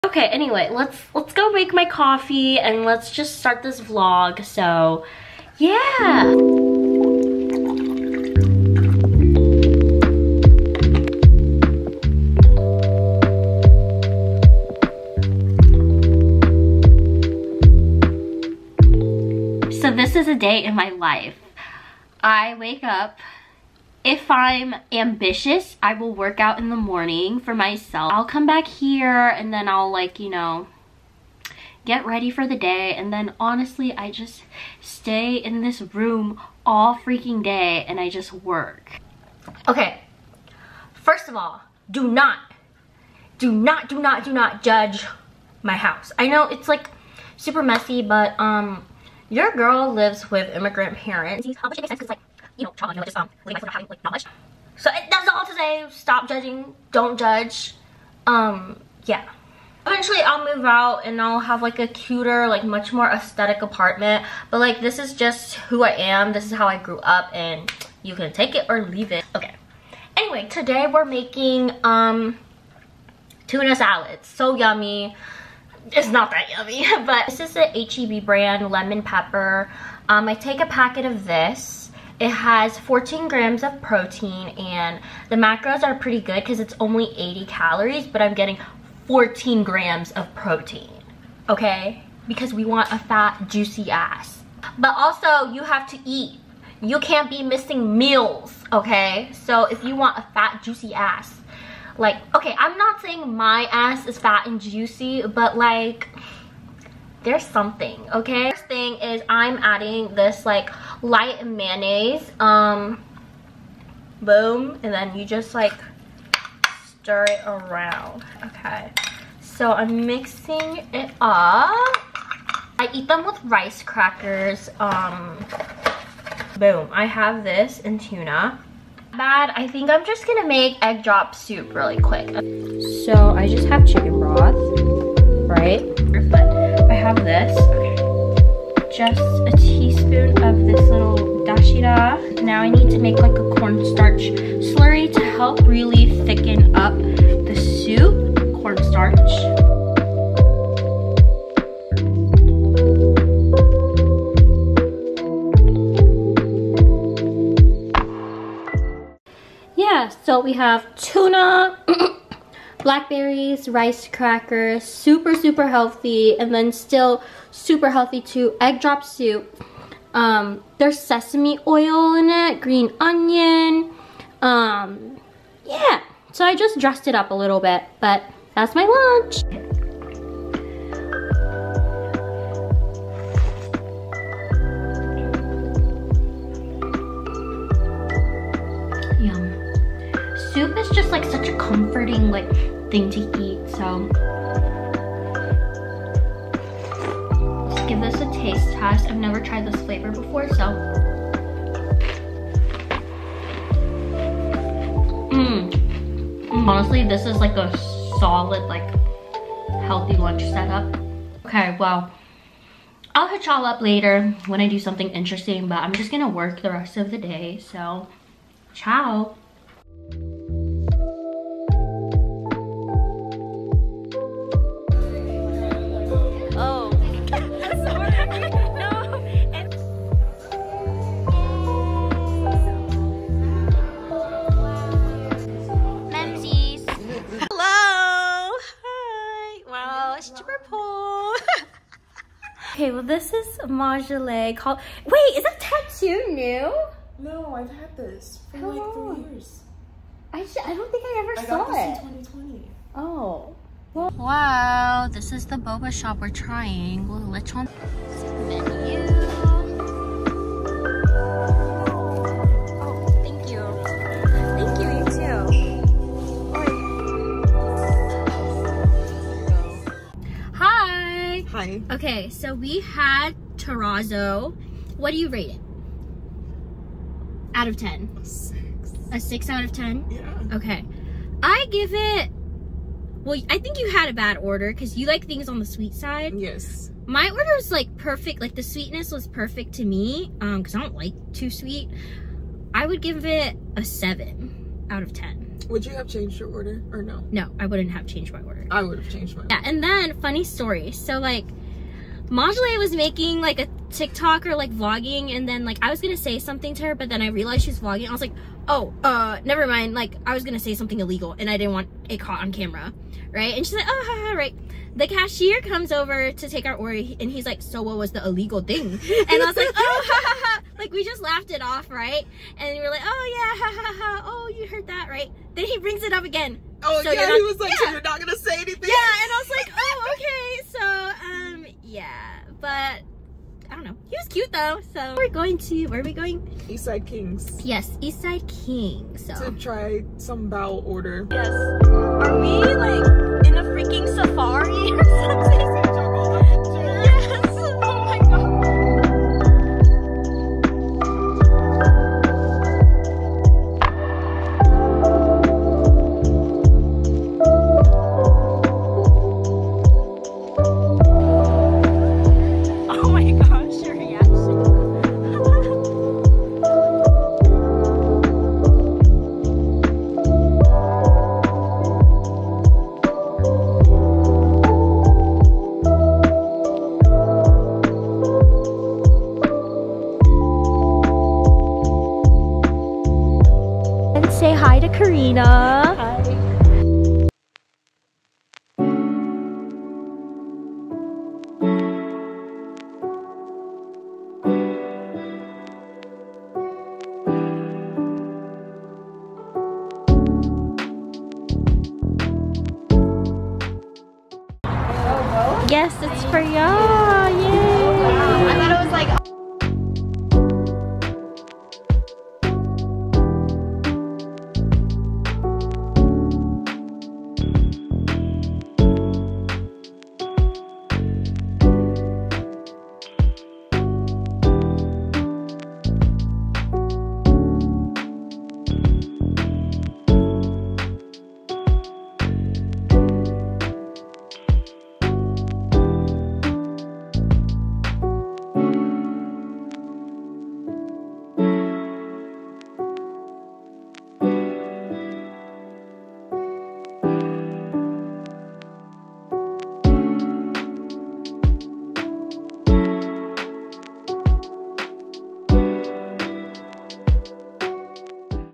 GB Diaries | ep 2 - lunch break, celebrating friends, trying a new boba spot + yummy hotpot
Hi friends!

I'm so excited to share another vlog with you all again! This weekend was so eventful with a short day trip to Austin, showing yall my lunch recipes and celebrating Vivian + Angela's birthday!